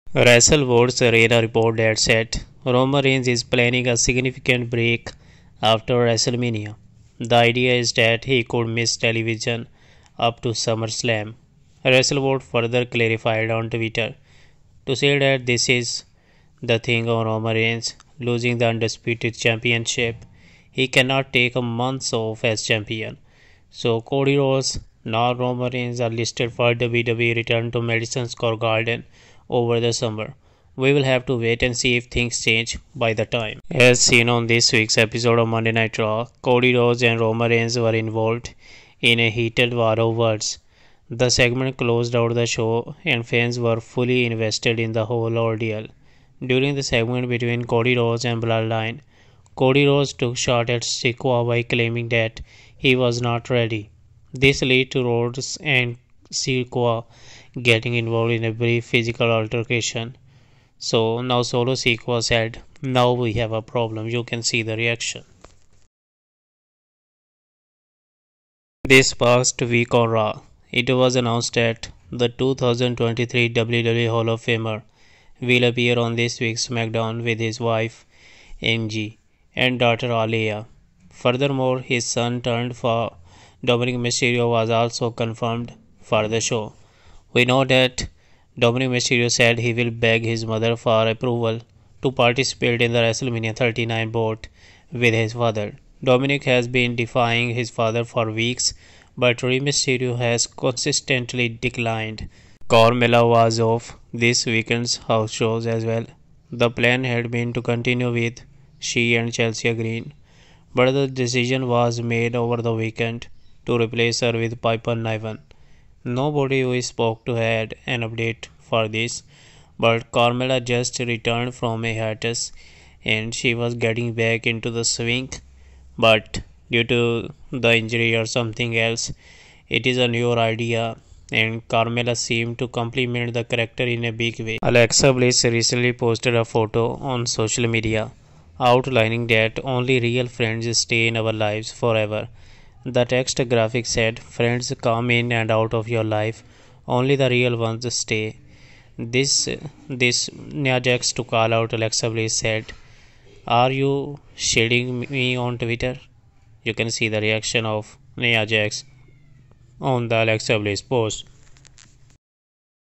Aliyah Returns on SMACKDOWN • Solo Sikoa is Ready • Roman Reigns Dropping Titles • Carmella Update..
1)
Carmella was off this weekend’s house shows as well. The plan had been to continue with she and Chelsea Green, but the decision was made over the weekend to replace her with Piper Niven. Nobody we spoke to had an update.
2)
Wrestle Votes ran a report that said Roman Reigns is planning a significant break after WrestleMania. The idea is that he could miss television up to SummerSlam.

Wrestle Votes further clarified on Twitter to say that this is contingent on Roman Reigns losing his Undisputed WWE Universal Title. Of course, he can’t take months off as champion.
3)
This past week at RAW, it was announced that the 2023 WWE Hall of Famer, will appear on this week’s SmackDown with his wife Angie and daughter Aliyah. Furthermore, his son-turned-foe Dominik Mysterio was also confirmed for the show.
4)
Alexa Bliss recently posted a photo on social media, outlining that only real friends stay in our lives forever. The text graphic said: “Friends come in and out of your life, only the real ones stay.” This prompted Nia Jax to call out Alexa Bliss.
5)
Following the conclusion of this week’s episode of Monday Night RAW, Solo Sikoa took to Twitter and called out The American Nightmare by making it clear he has a problem with Rhodes.